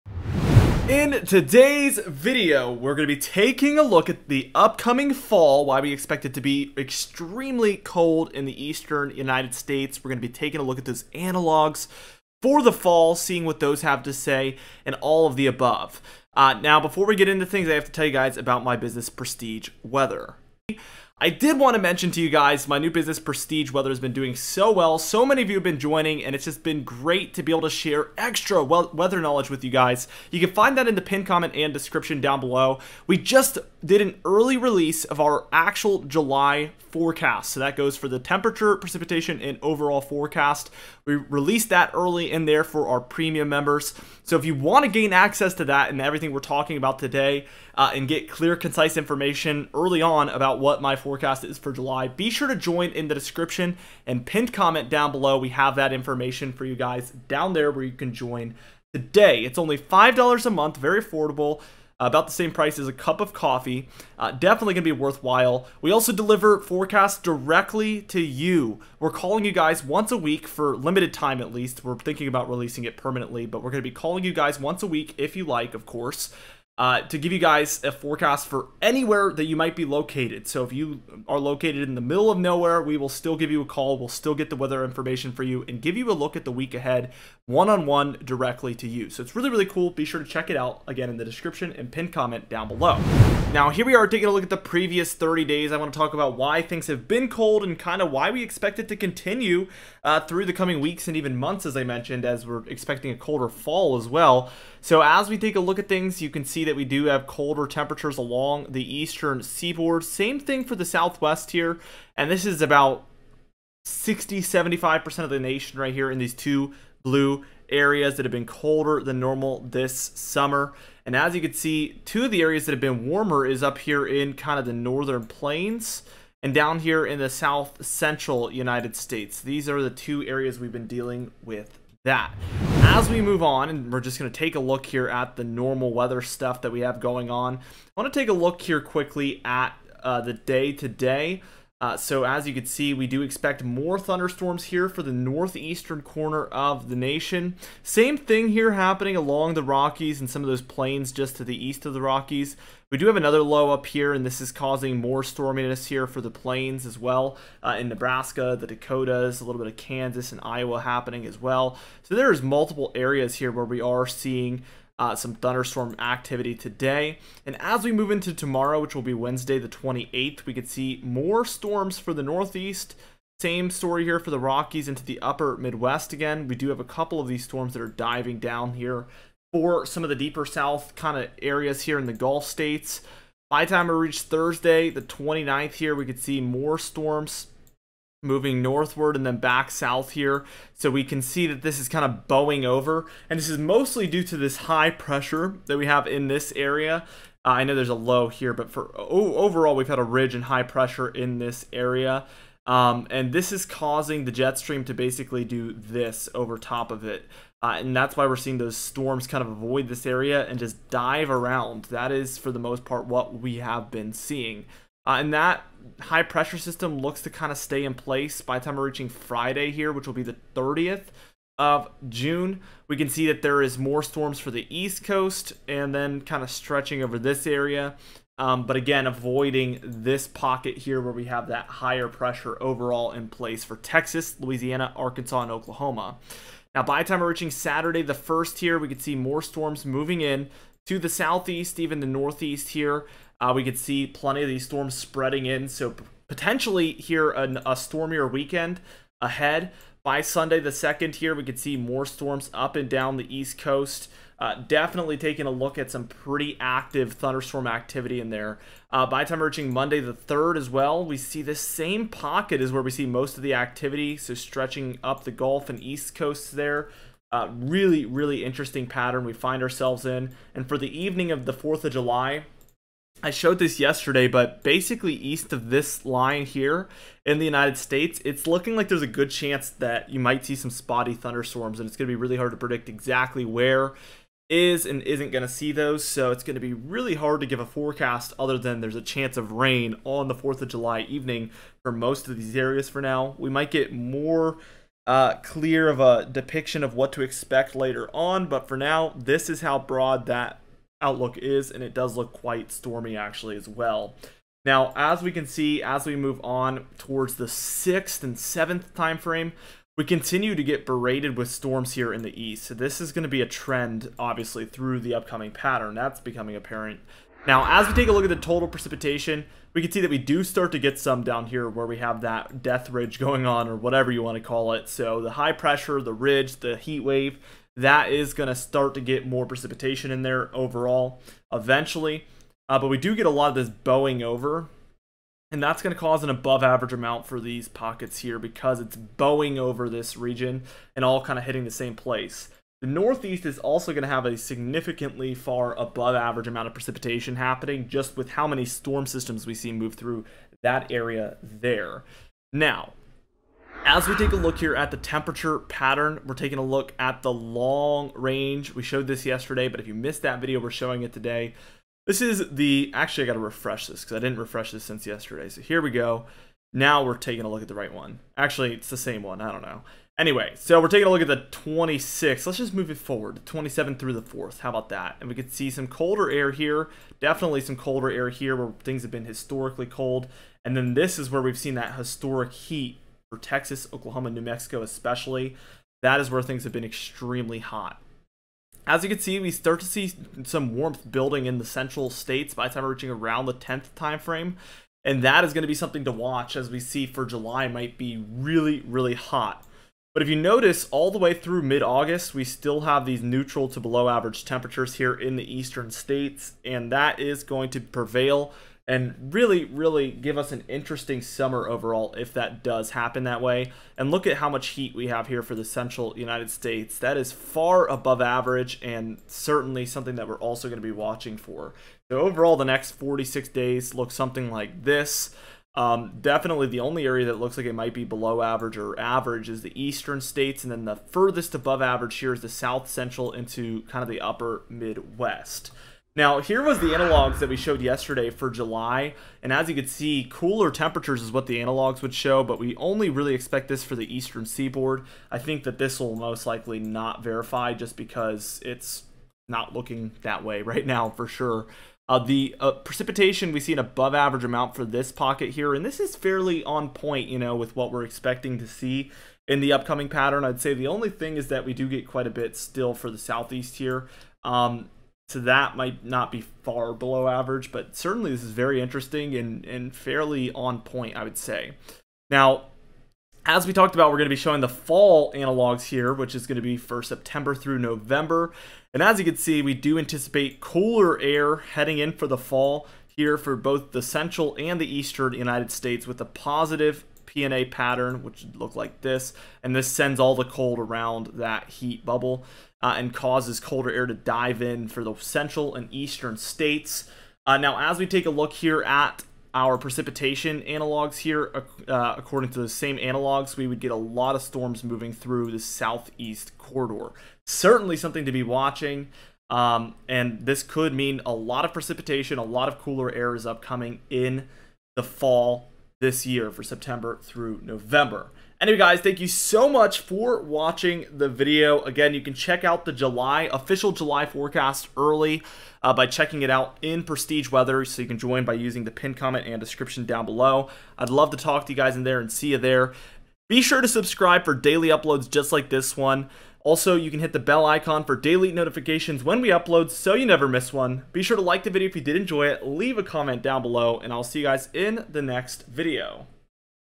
A Look at the Upcoming FRIGID Fall?! - Fall 2023 Analogs
Our Awesome Direct Weather Store!

**ALWAYS BE SURE TO SEEK OFFICIAL GUIDENCE FROM THE NWS AND OR THE NHC IN THE CASE OF A DANGEROUS OR LIFE THREATENING EVENT**